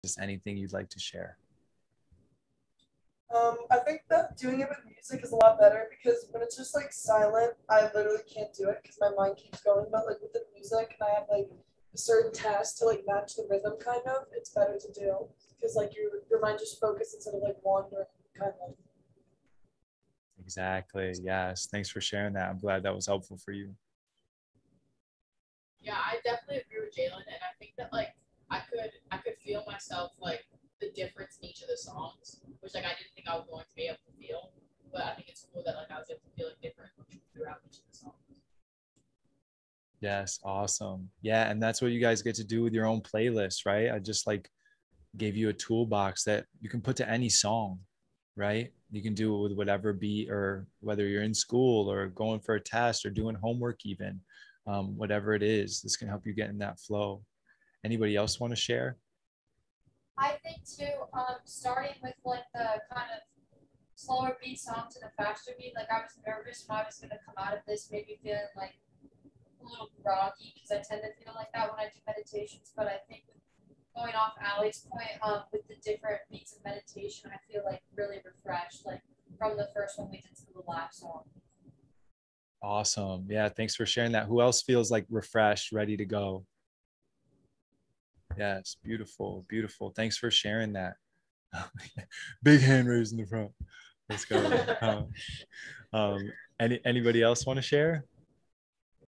Blair Academy Breathwork With Ben Team Training Feedback
Blair Academy basketball players share about their experience after a breathwork meditation with Ben.